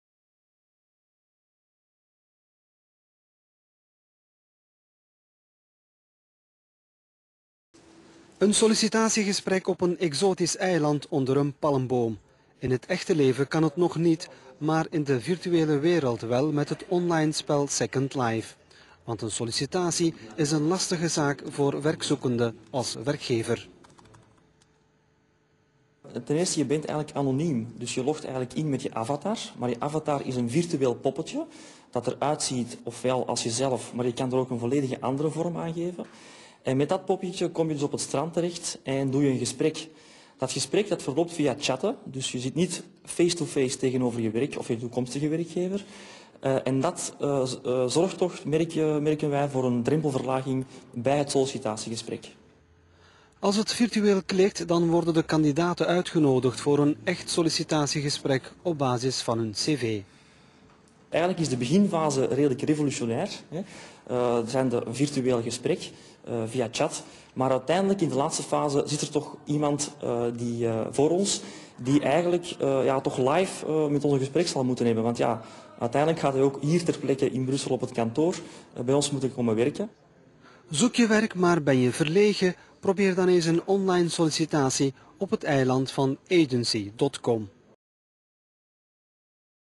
interview on television
a guy talking on television about Second Life